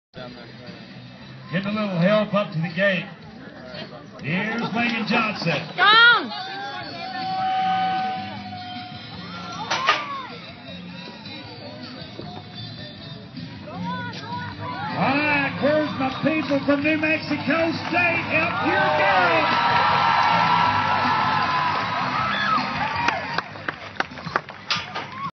Long round las cruces nm rodeo 2
Nellie at the long round for the #2 rodeo for nmsu placed 2nd